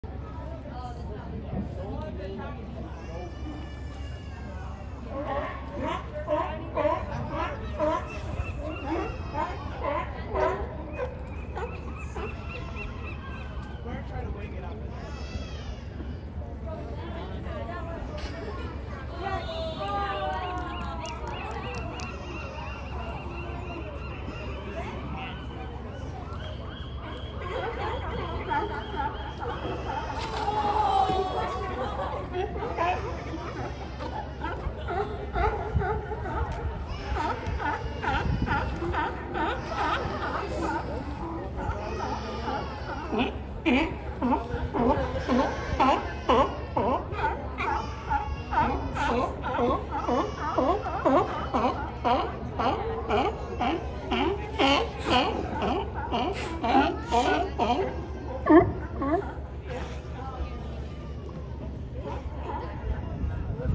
Pier 39 Sea Lions in San Francisco
Sea Lions on Pier 39 in San Francisco, California, on Monday, April 18, 2022.